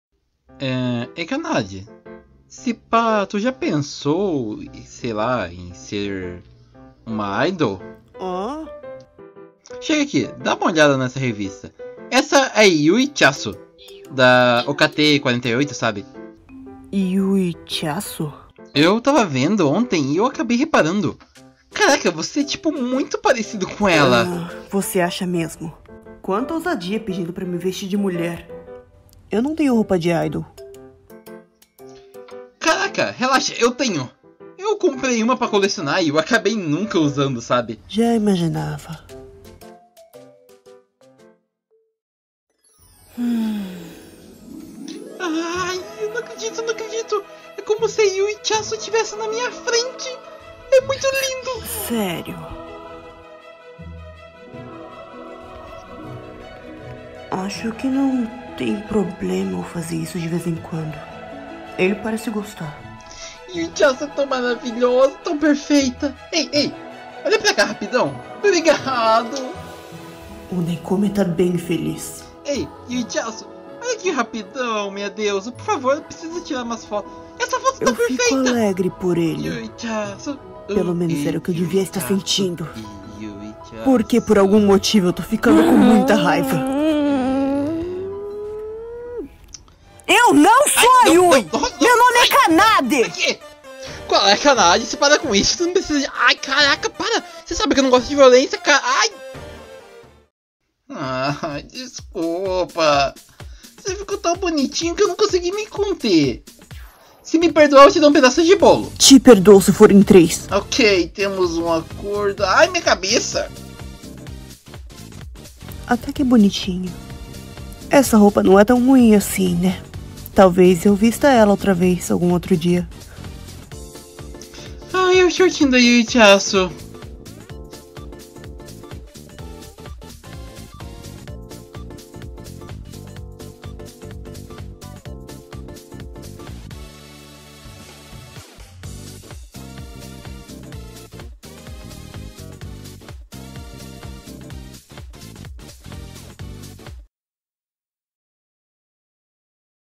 SERÁ QUE VOCÊ FICA BONITO COMO IDOL ? (Manga Dublado)
|Novo Episodio dessa Serie então bora aproveita?
Eae oque acham q o Kanadde seria uma boa Idol. ?  , oque deveria usar da próxima vez?

Manga - Tomodachi ga Skirt haite mitai tte Itte kita Ken
Canal da Criadora   @寺井赤音-j9x
Participação :
Maruyuki Tenshi - Kanada
Eu - Nekome (